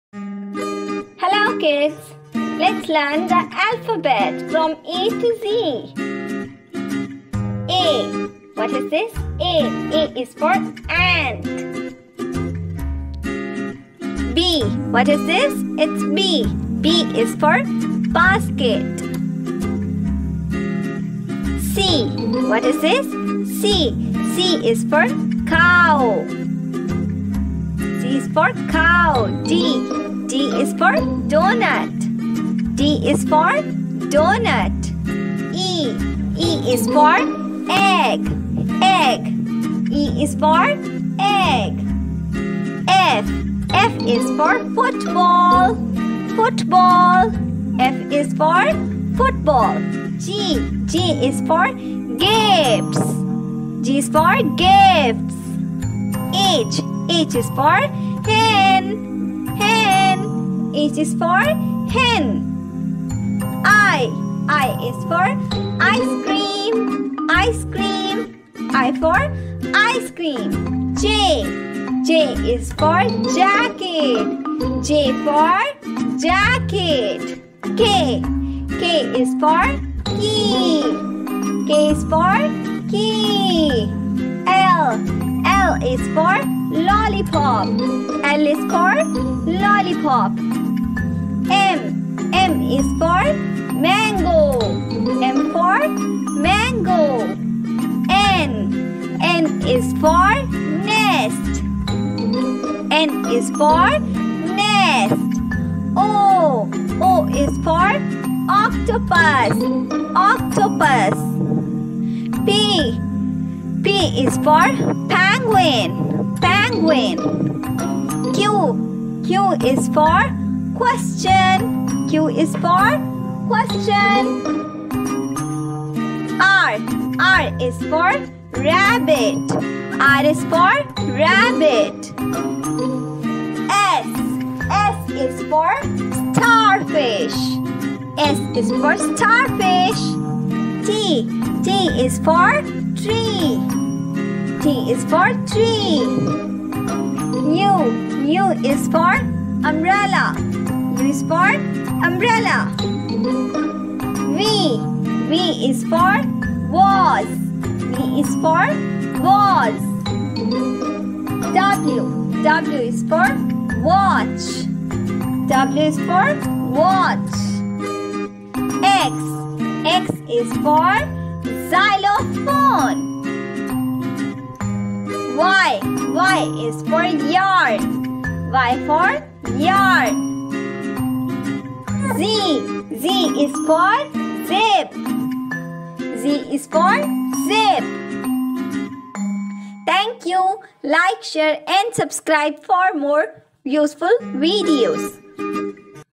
abcd abcd, a for apple b for ball, abc alphabet learning, a to z alphabets, educational video
here are the alphabets with different colorful objects that will help your kids in learning and memorizing these 26 alphabets. hope this video will make your way easy and your kids will learn and recognize the alphabets easily with the colorful pictures and music as well.